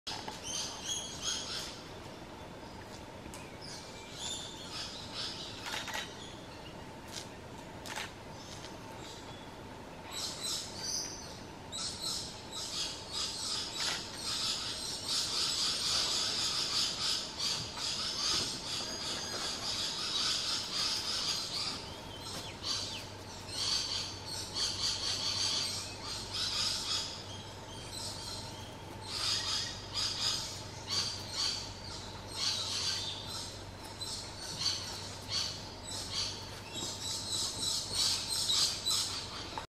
female cassowary approaching male with chick
Sometimes a cassowary sits down when being approached by another one,
but, unless it is a female who wants to mate, they always get up and run away when the other one gets too close for comfort.